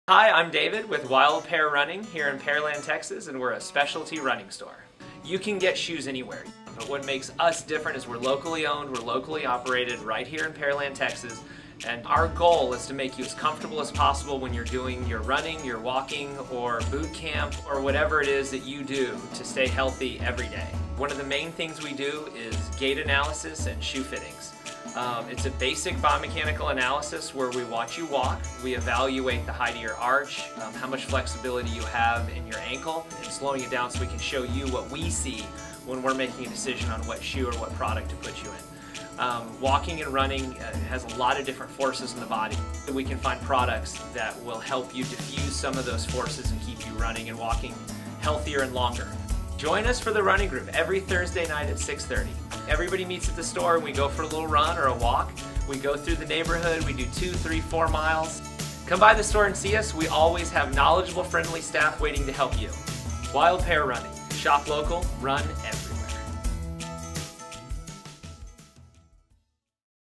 Wild Pear Running
9330 Broadway St.  |  Pearland Texas 77584

Expert Running and Product Advice. With more than 25 years of shoe fitting and running experience, Wild Pear Running is a one-stop shop for all of your Running and fitness needs. Whether you are training for a fall marathon or just staying fit this summer we have the footwear and products you will need.

Created by the Houston Community Newspapers video team
Co-Produced, Directed and Edited by Megan O'Sullivan